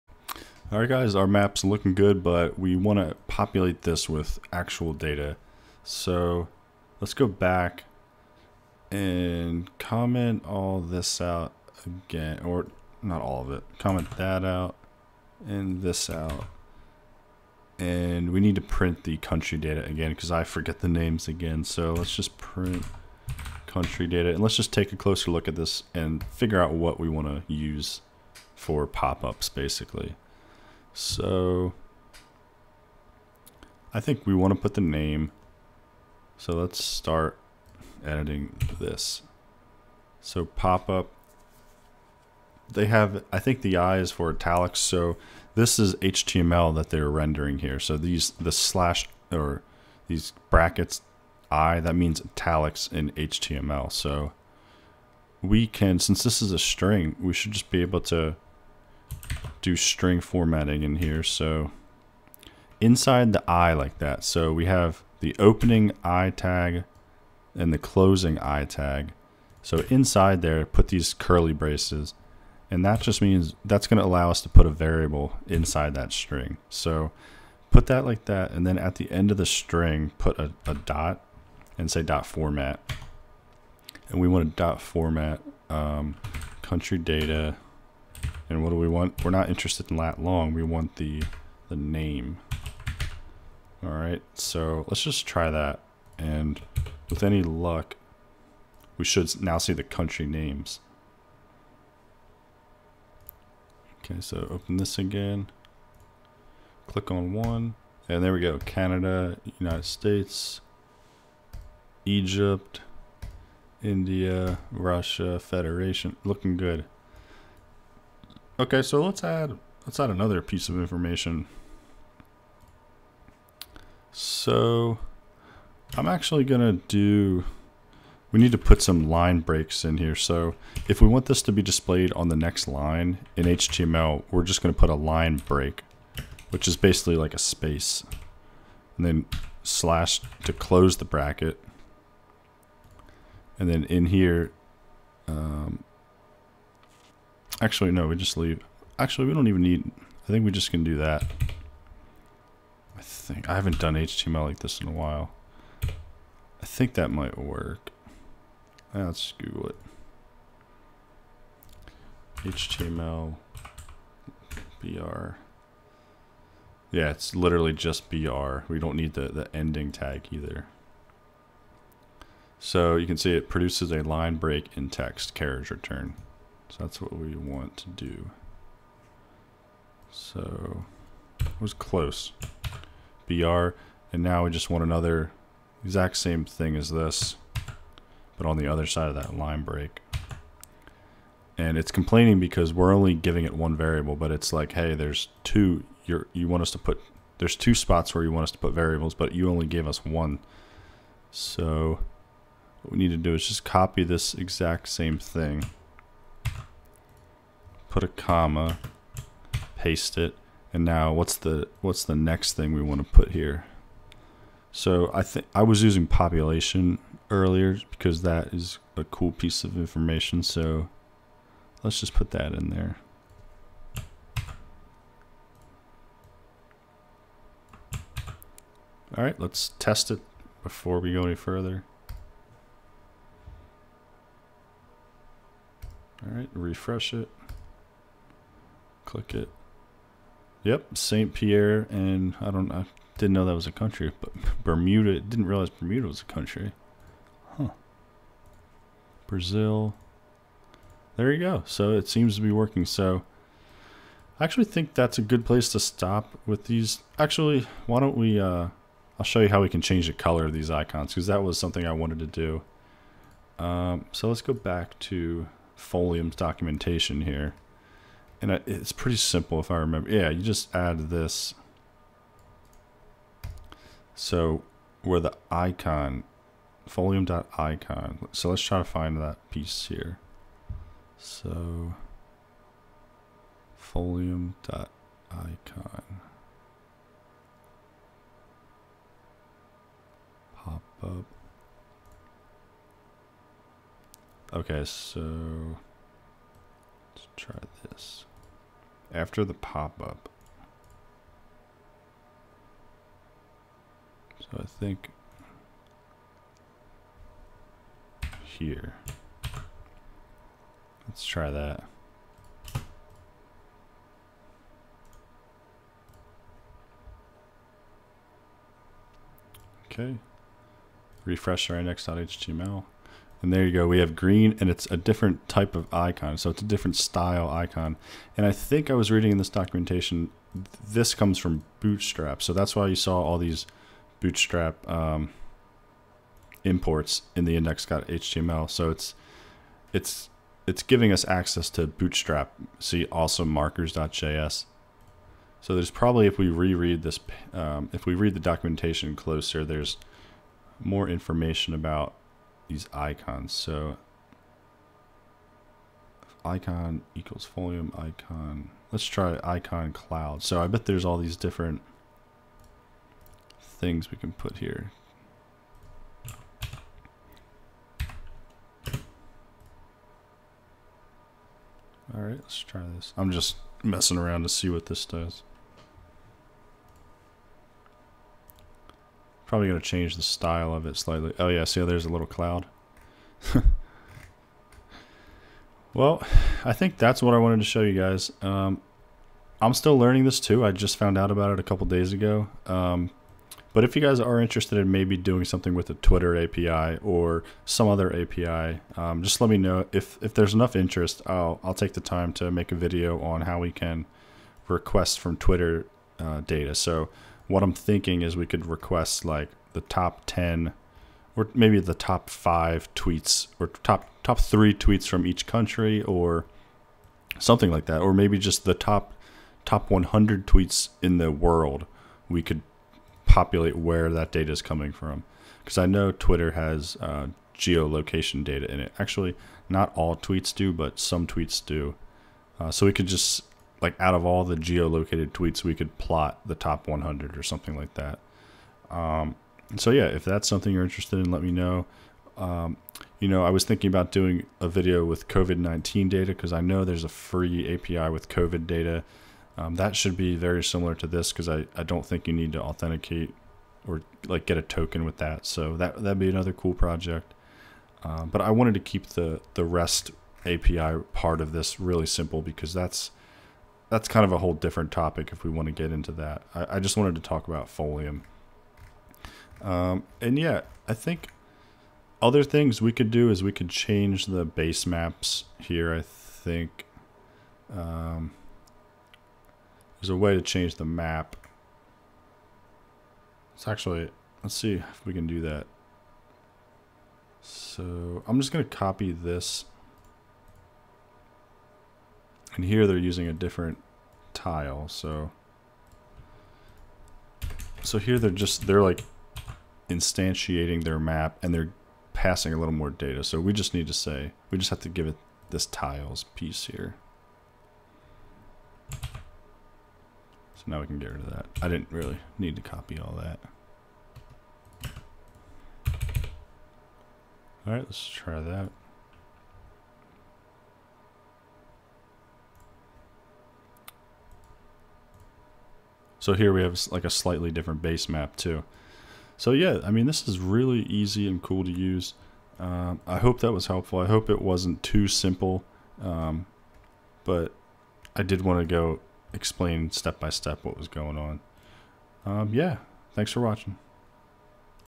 6 - Adding Pop Up info based on API response - Web Mapping with Python Folium Basics
Learn how to use the Python library Folium to make Web Maps easily!